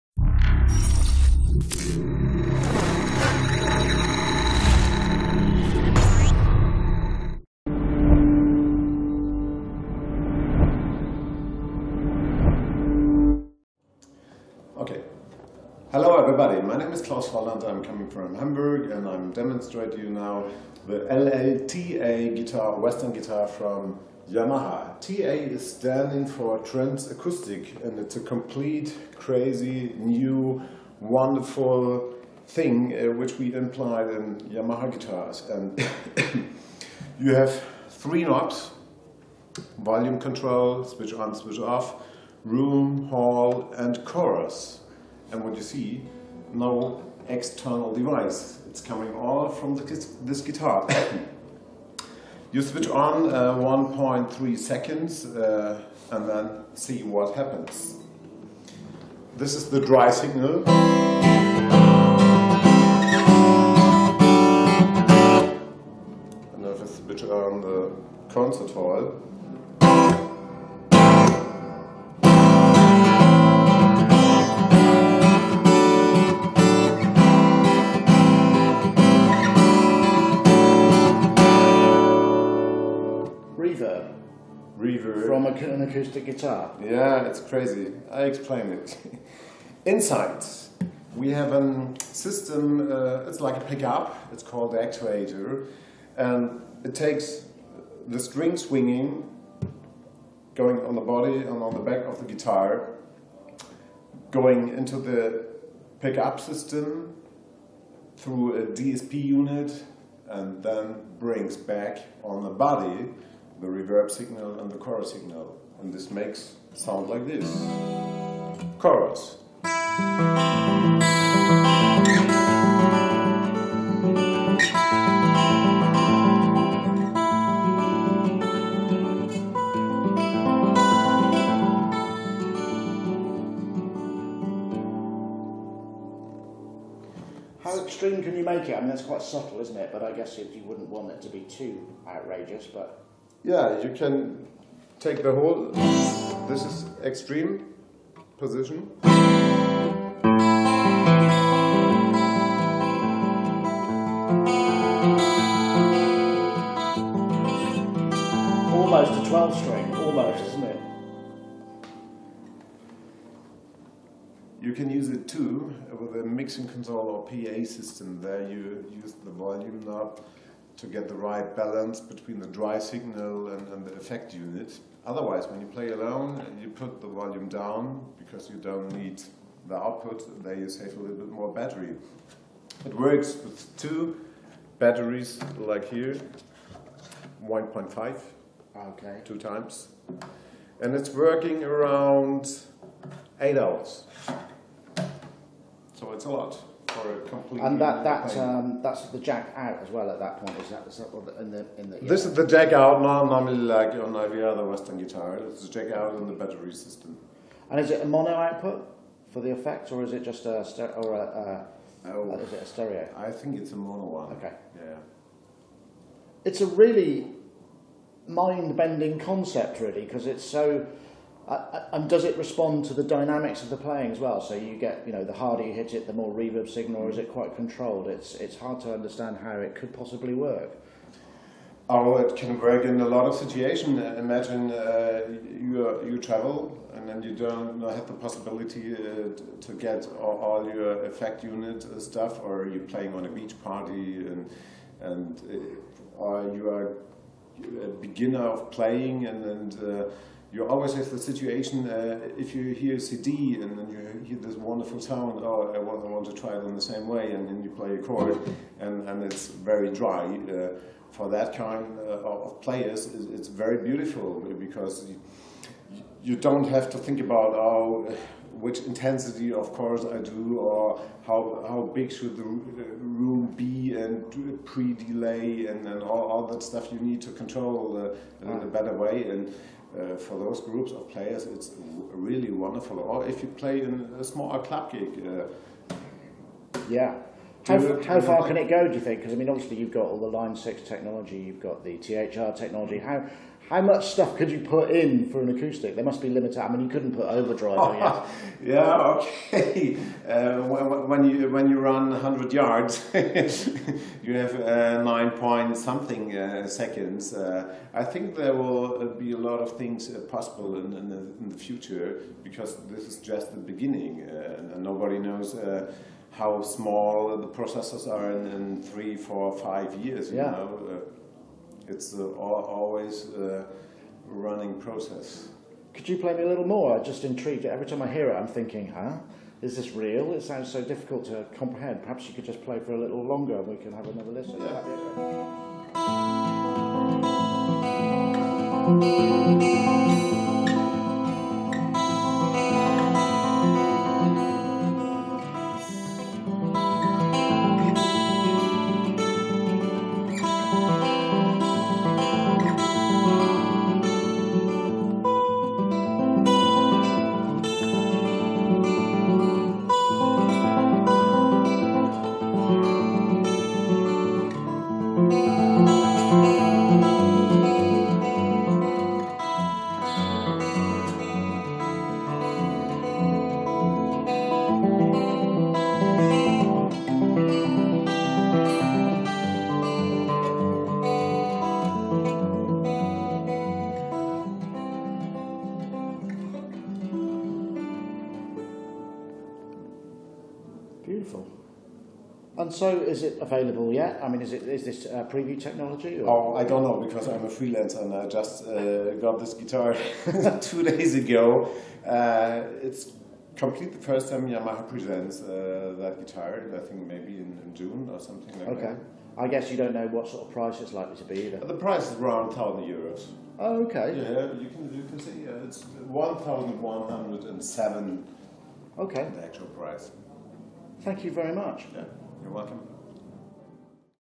MESSE 2016: Yamaha's New TransAcoustic Guitar is CRAZY!
Reverb and Chorus within the body of your acoustic guitar!!